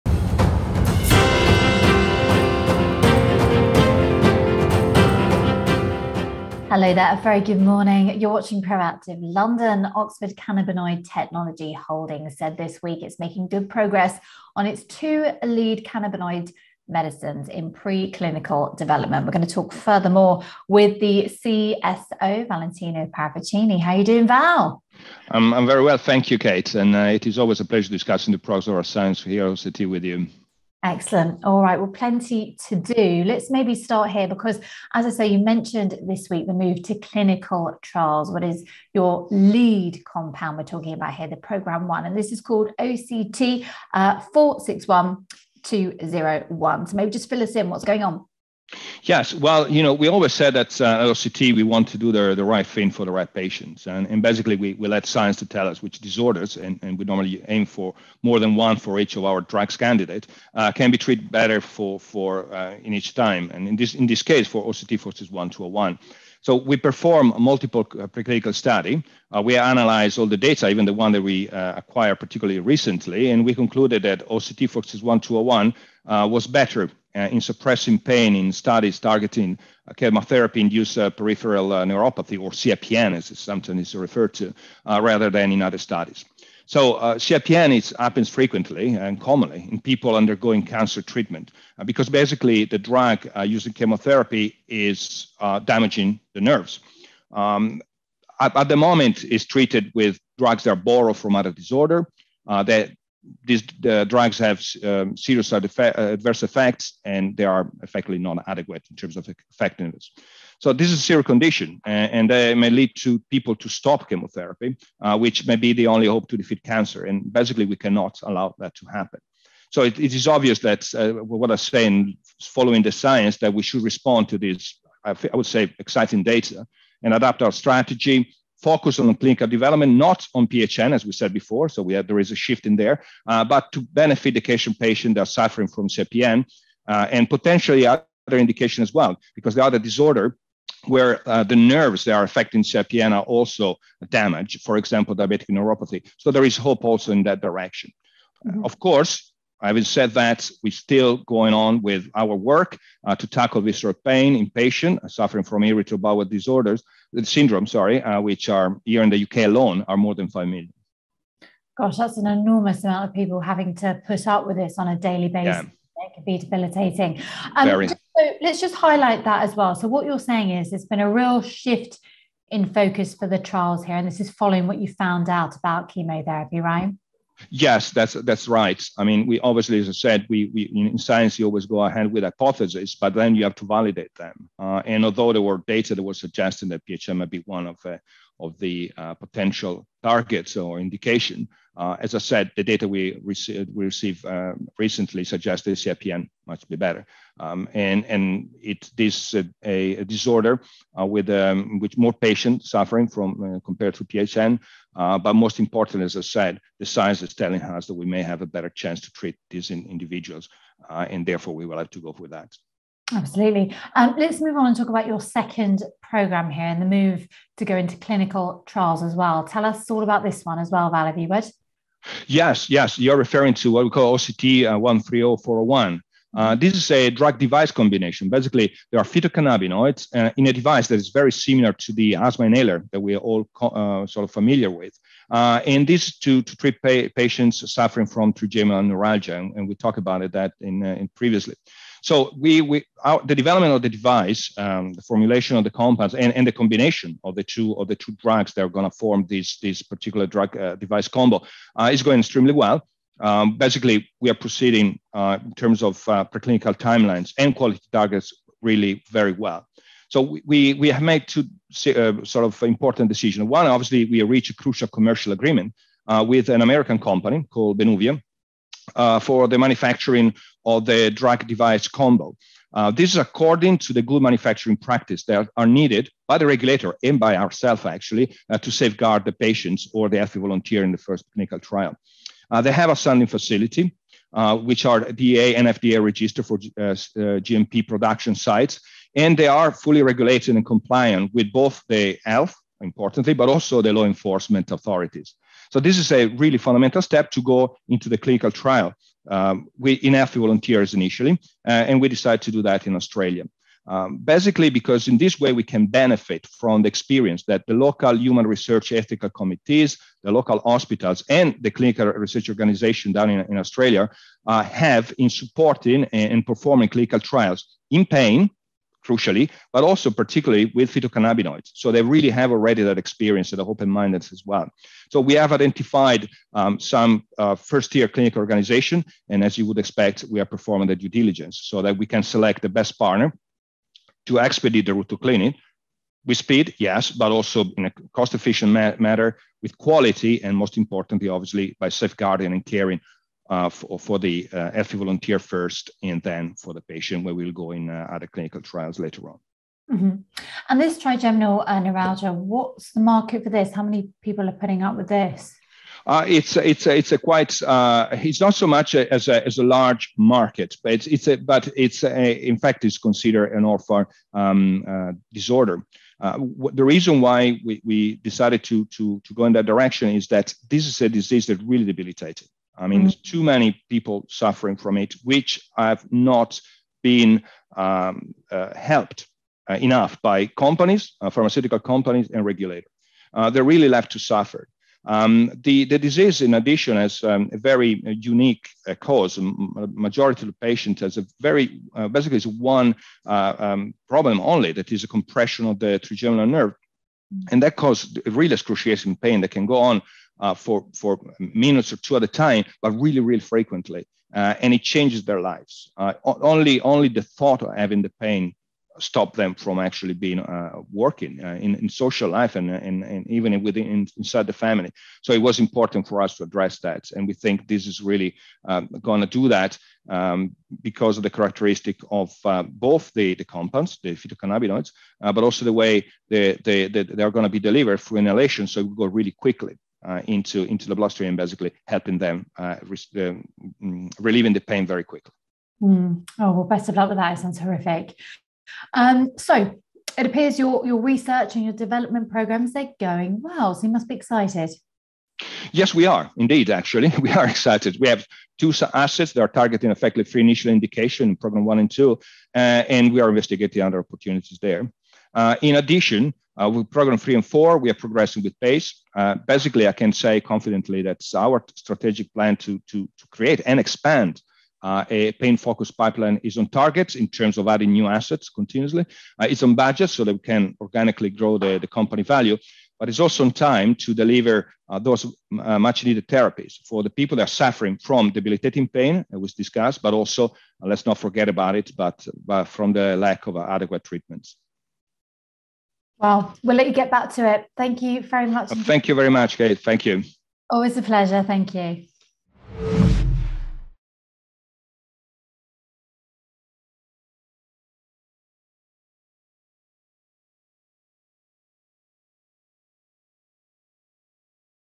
Oxford Cannabinoid's CSO explains why cancer patients suffering CPN are now its focus
Oxford Cannabinoid Technology’s CSO Valentino Parravicini talks to Proactive about why it has switched focus for oct461201 from post-herpetic neuralgia (PHN) to chemotherapy-induced peripheral neuropathy or CPN.

After analysing data from multiple studies, he says the conclusion was that the compound was better in suppressing pain in CPN sufferers and less effective in PHN, hence the change.

CPN occurs frequently in people undergoing chemotherapy for cancer because of damage to nerves, with the current standard of care treatments also having serious side effects.